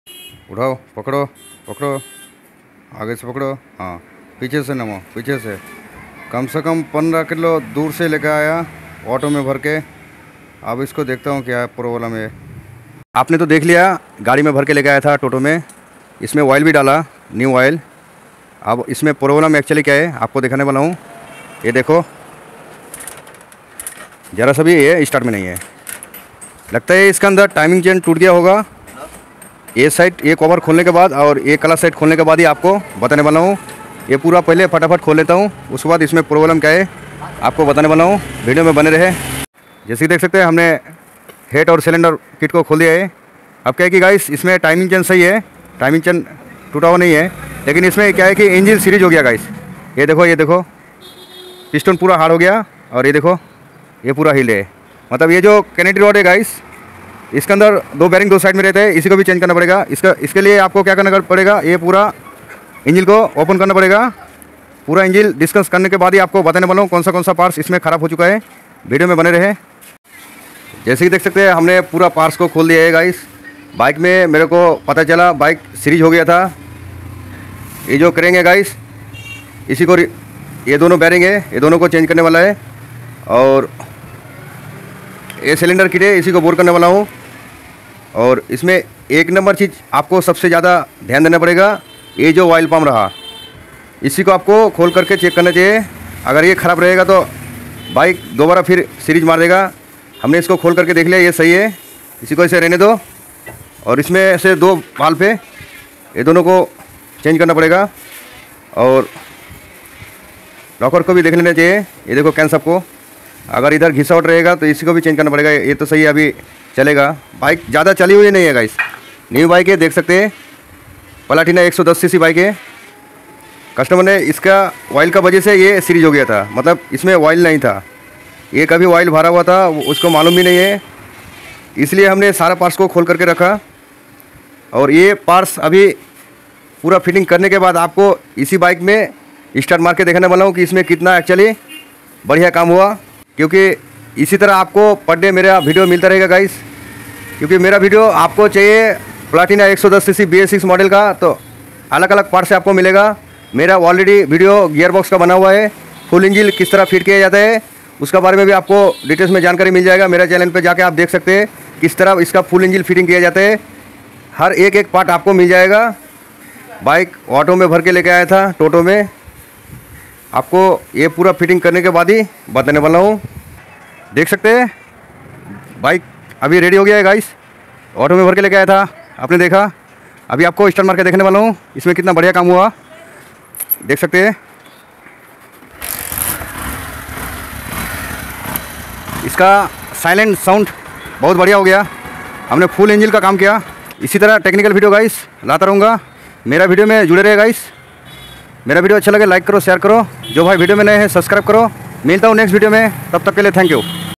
Bajaj Platina 110 bs6 starting problem Angel series
Bajaj Platina 110 bs6 engine series program starting

Angel oil nahin tha isliye engine series Ho Gaya iska problem